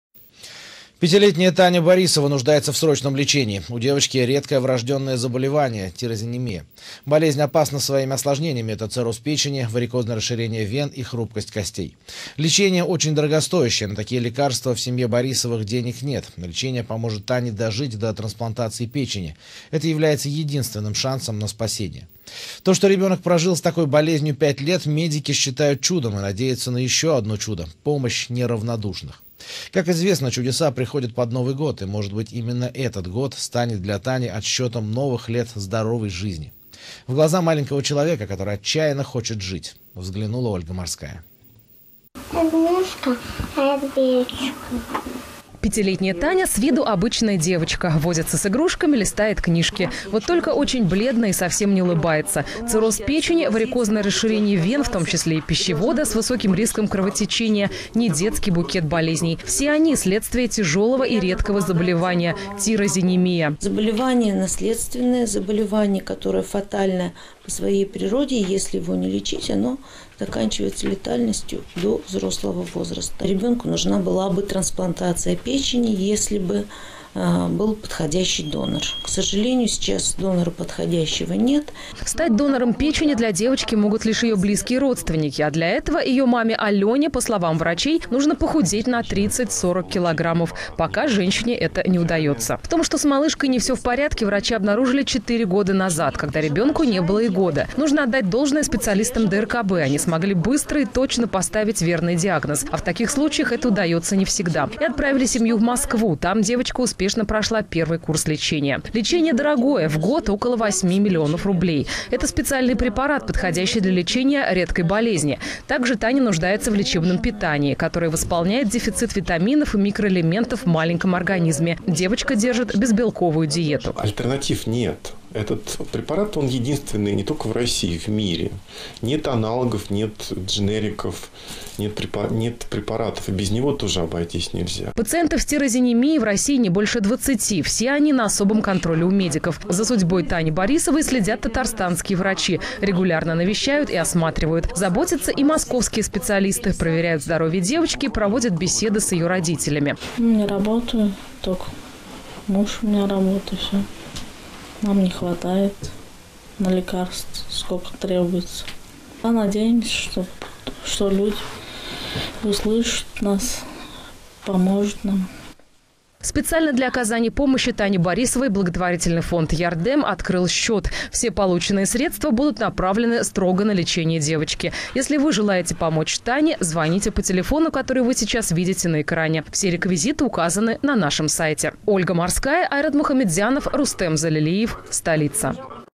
СПАСТИ ЖИЗНЬ
5-летняя Таня Борисова нуждается в срочном лечении: у девочки редкое врождённое заболевание - тирозинемия. Болезнь опасна своими осложнениями: это цирроз печени, варикозное расширение вен и хрупкость костей. Лечение очень дорогостоящее. На такие лекарства в семье Борисовых денег нет. Лечение поможет Тане дожить до трансплантации печени, это является единственным шансом на спасение. То, что ребёнок прожил с такой болезнью 5 лет, медики считают чудом. И надеются на ещё одно чудо - помощь неравнодушных. Как известно, чудеса происходят под Новый год, и может быть, именно этот год станет для Тани отсчетом новых лет здоровой жизни. В глаза маленького человека, который отчаянно хочет жить, взглянула Ольга Морская.